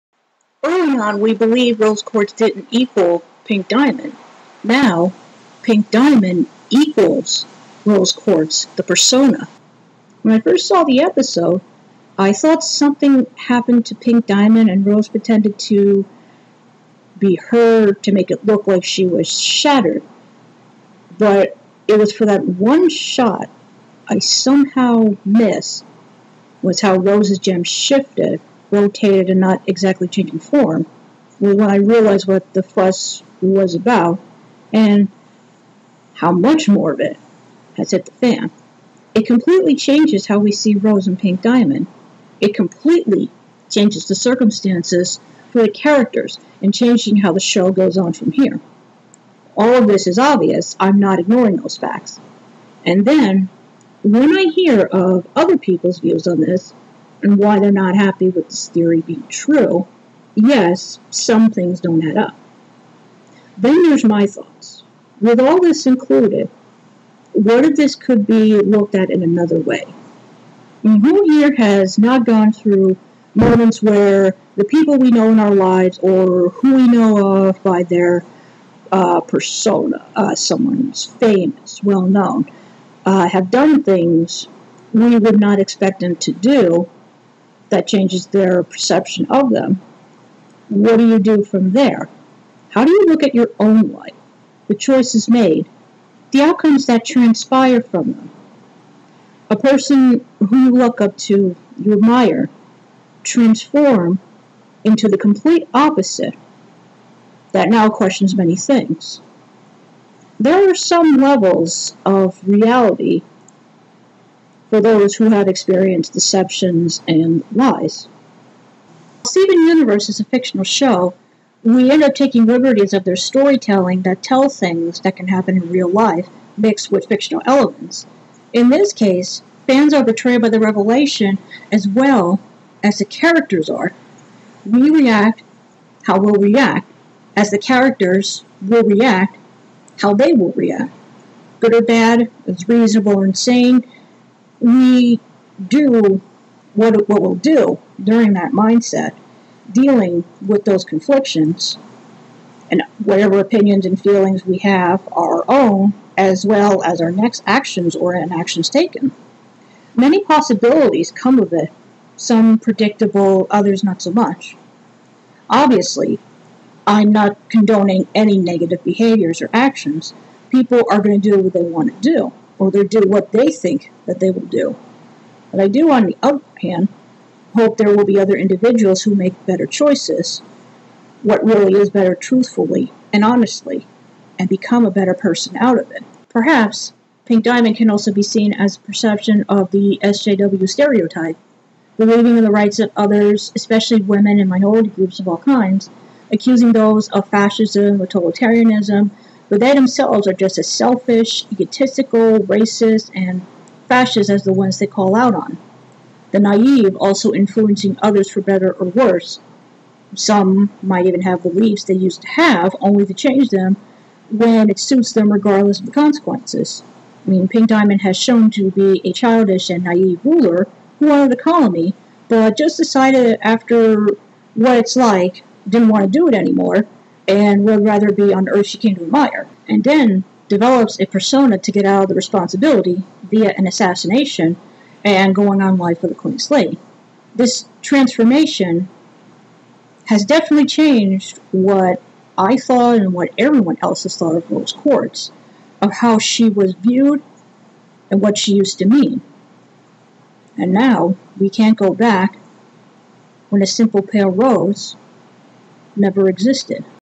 STEVEN UNIVERSE: My Pink Diamond Commentary
A few things I pondered over after the latest Steven Universe episode. They're my comments/insights and not meant to harm others or disrespect.
Audio quality isn't as great; I'm still working on editing skills.
The image used is a drawing I did a while back, it can be found on my Deviant Art